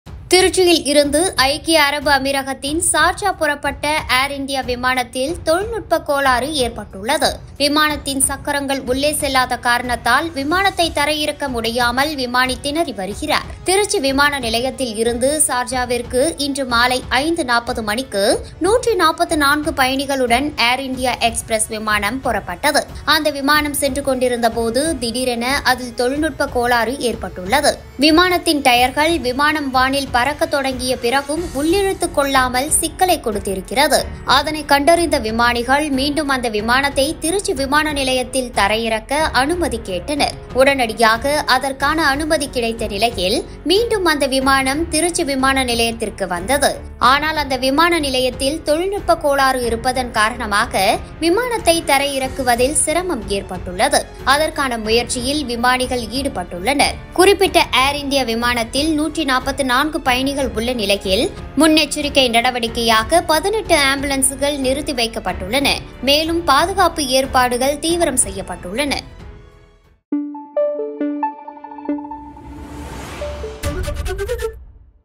Trichy Flight Landing Issue | தரையிறங்க தவிக்கும் விமானம்..திக் திக் நிமிடங்கள் பரபரப்பில் திருச்சி
Trichy Flight Landing Issue | தரையிறங்க தவிக்கும் விமானம்141 பேரின் திக் திக் நிமிடங்கள் உச்சகட்ட பரபரப்பில் திருச்சி

Credits
Producer :
Script :
Editor :
Voice over :
Published by :